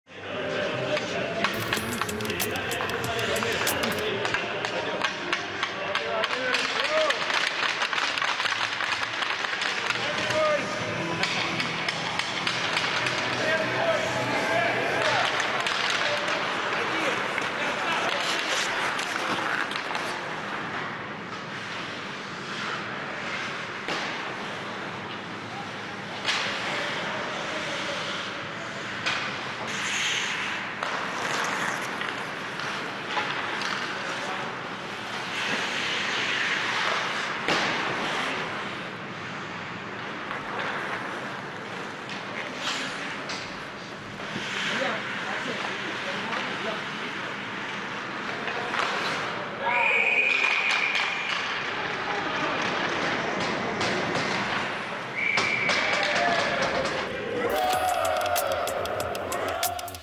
Russia's Putin and Belarus' Lukashenko play hockey after talks
Russian President Vladimir Putin and his Belarusian counterpart Alexander Lukashenko play ice hockey after holding diplomatic talks.